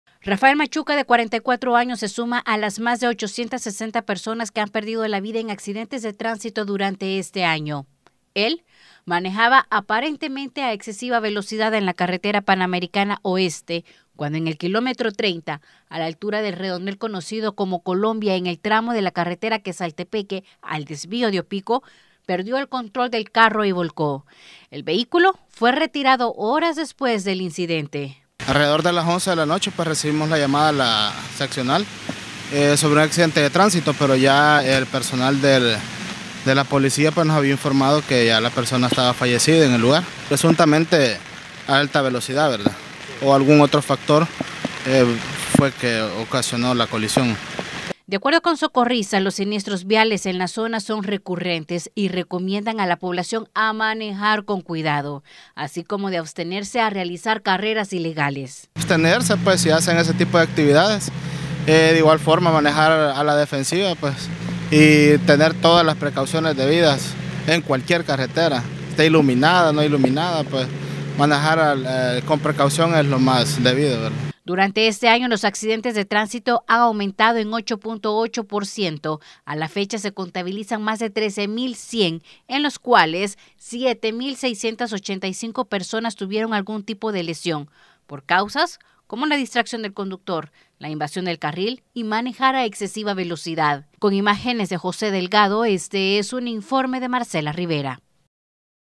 Un fallecido en accidente de tránsito
Una persona perdió la vida en un accidente de tránsito registrado en la carretera Panamericana Oeste. Autoridades contabilizan más de 13,000 siniestros que han dejado 7,685 lesionados.